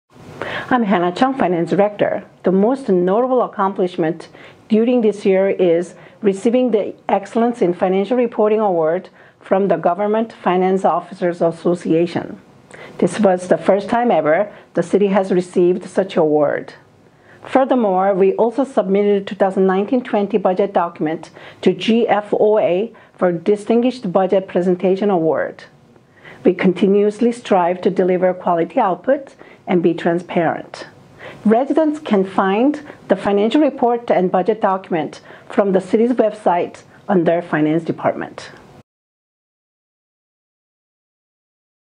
2019 Finance
Hannah Chung shares what occurred in the Finance Department in 2019 and looks at 2020.